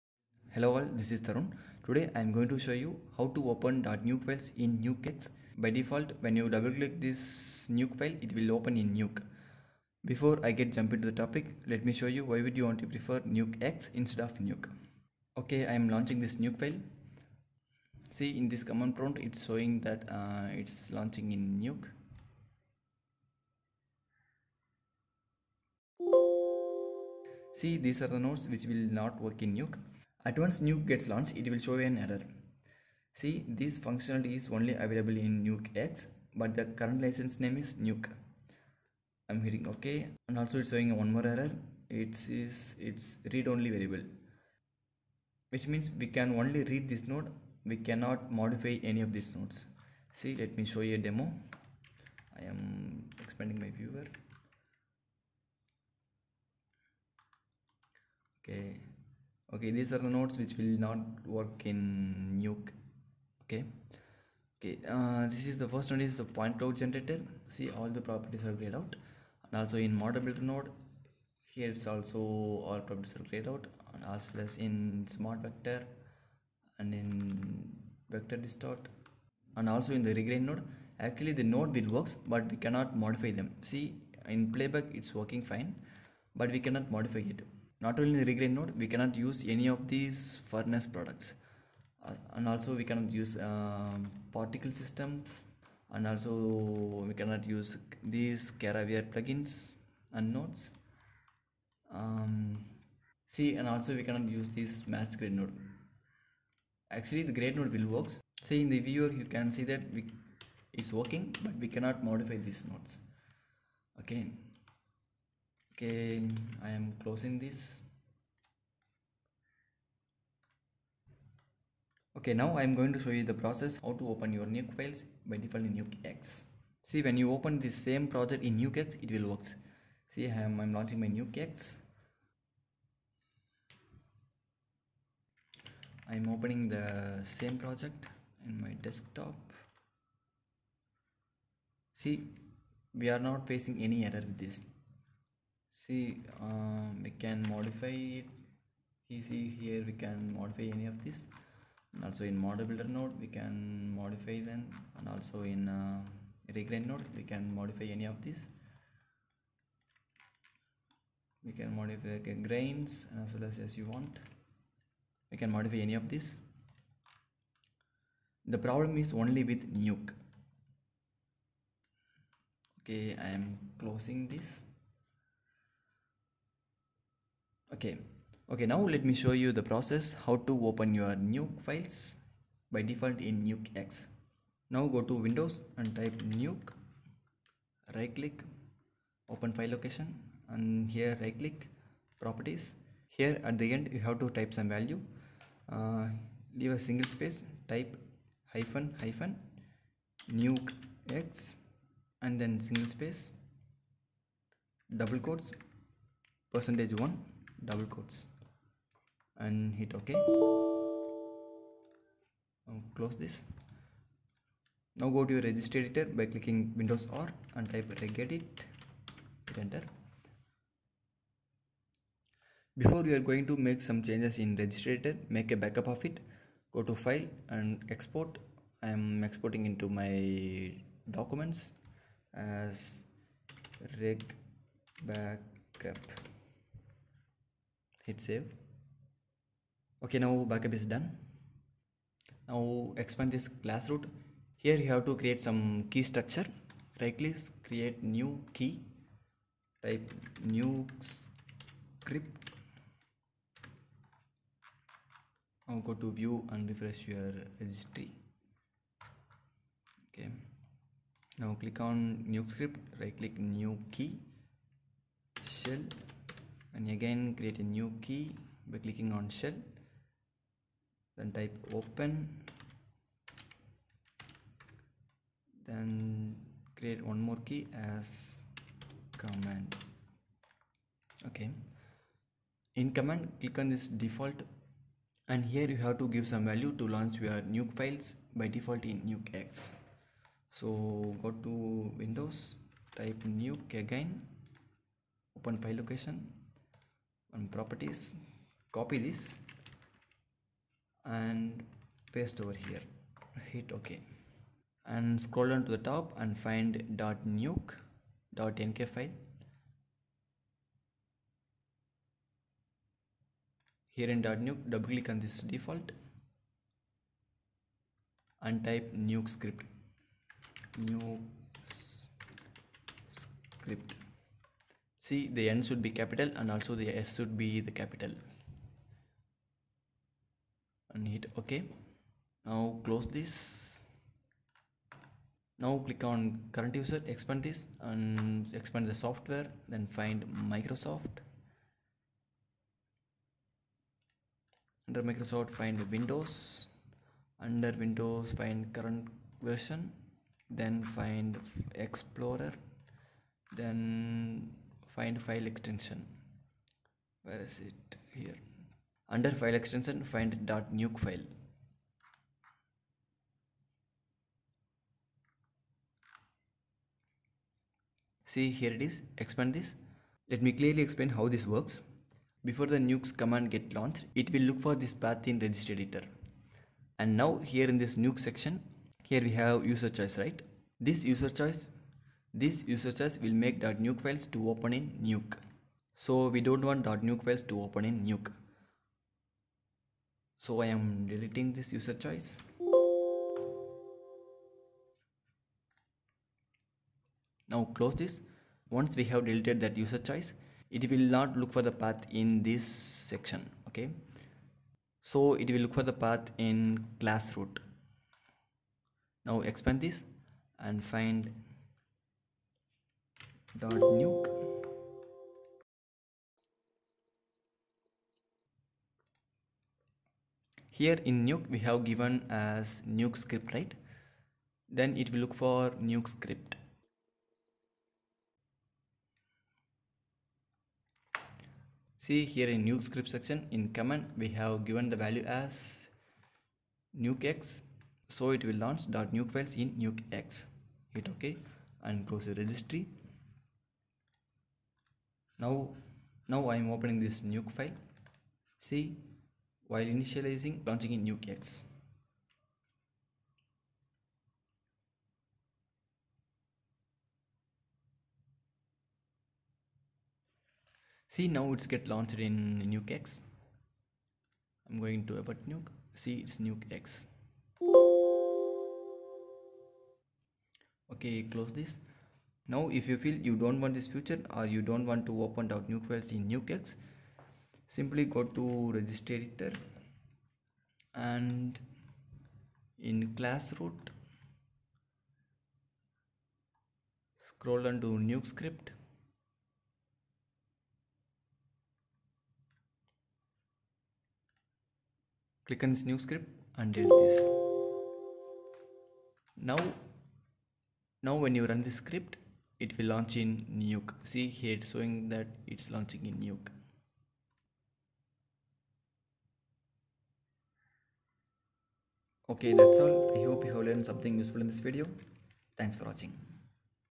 Open .nk project files, by default in nukeX
Usually .nk files will launch in nuke.
In this video, I have explained about how to open any nuke project files by default in nukeX.